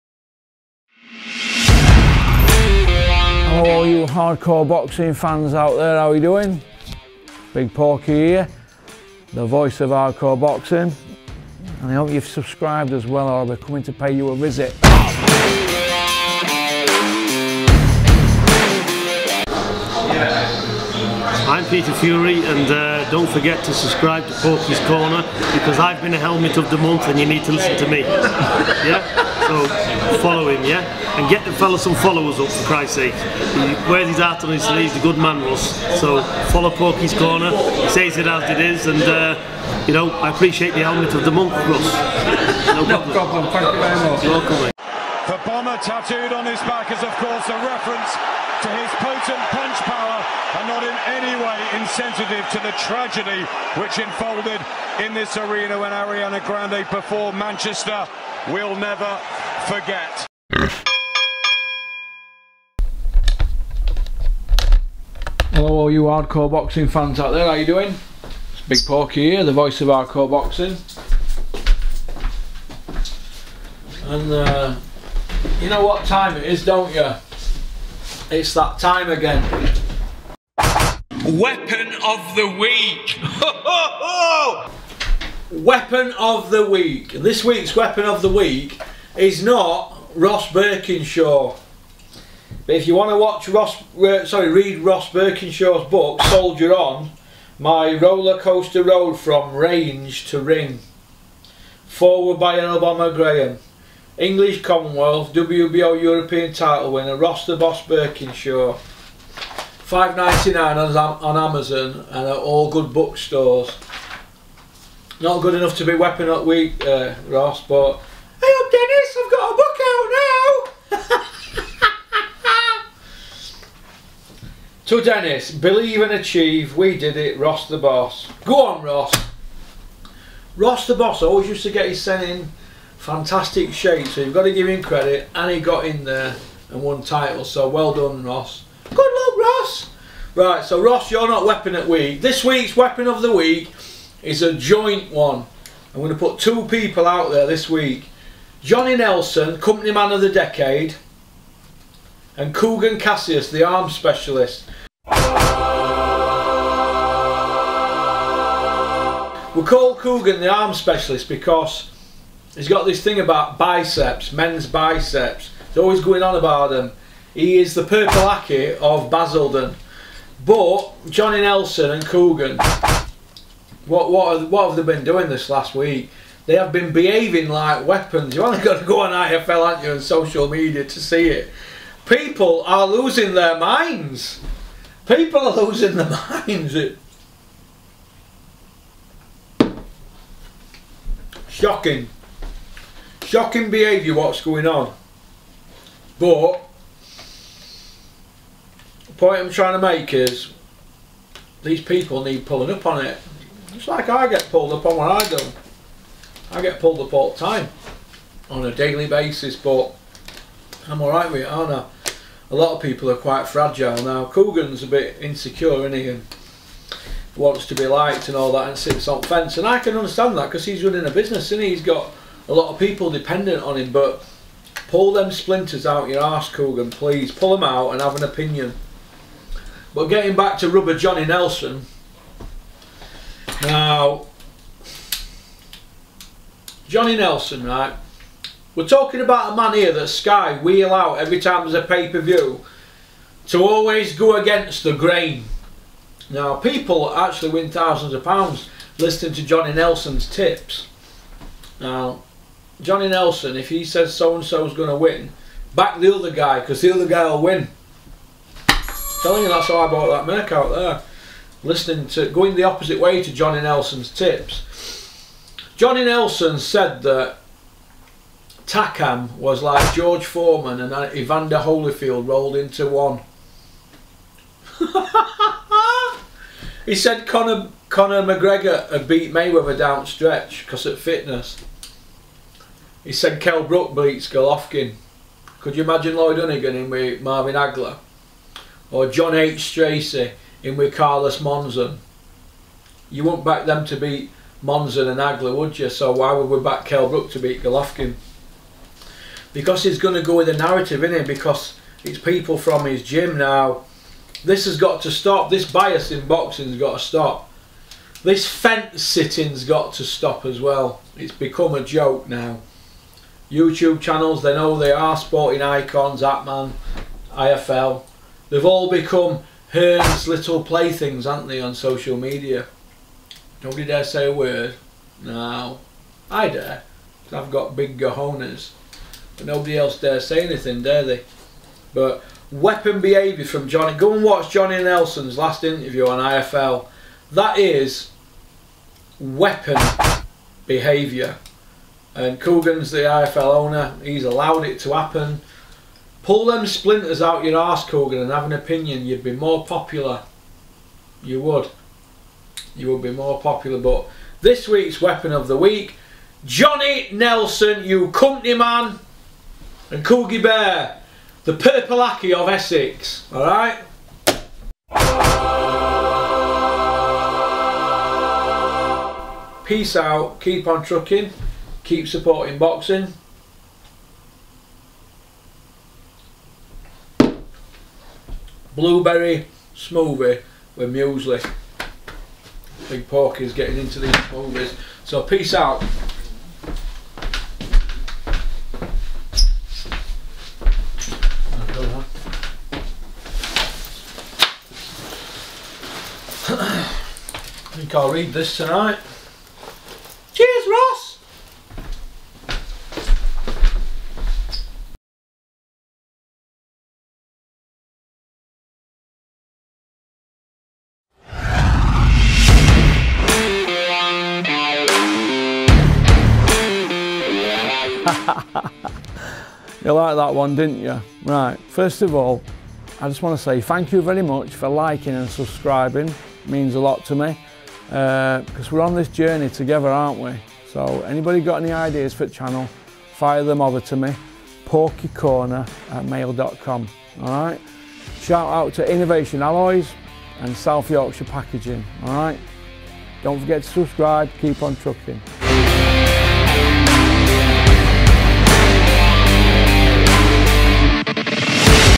Weapon of the Week Vol 13
It's the weekend, so that can mean only one thing - IT'S WEAPON OF THE WEEK!

Kugan Cassius and Johnny Nelson are in Porky's crossfire for failing to have an opinion with IFLTV. Also featuring Eddie Hearn, Purple Aki, Carlos Takkam, Ross Burkinshaw and others.